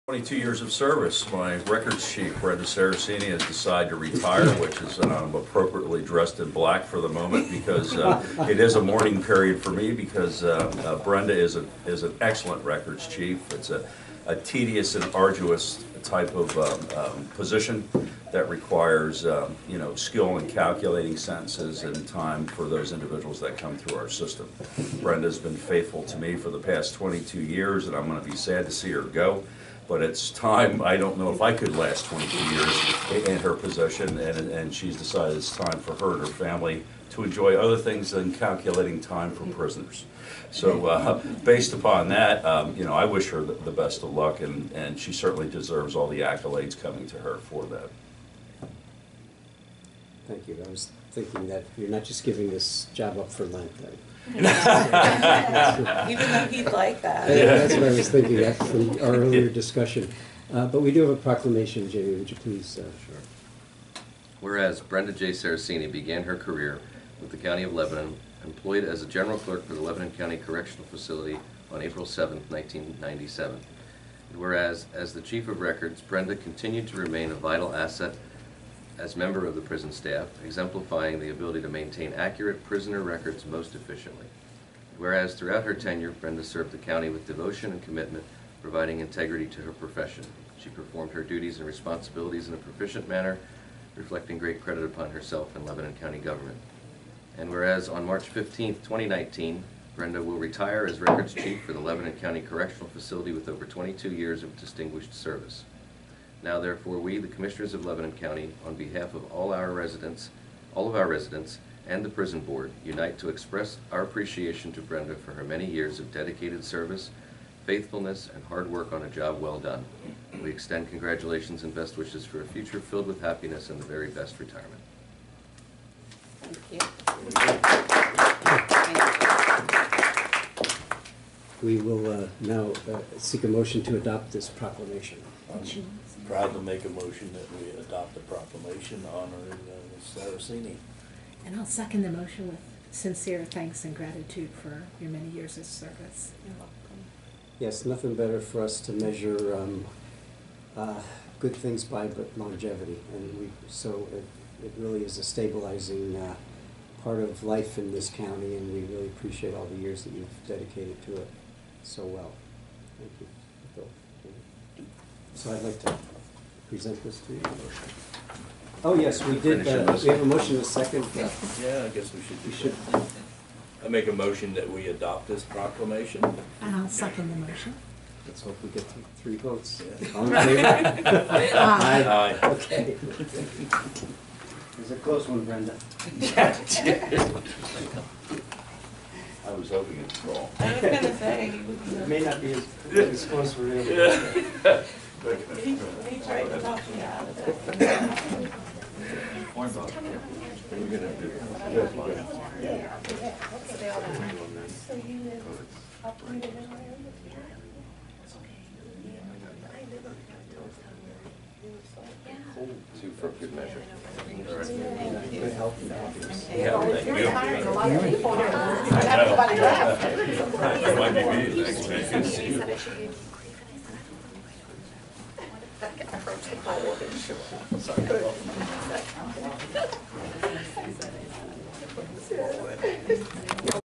3 7 19 Brenda Ceresini retires
Lebanon PA, 3.7.2019:  Brenda Ceresini retire with over 20 years tracking inmate expenses at the Lebanon County Correctional Facility.  Thank you for your years of dedicated service, Brenda.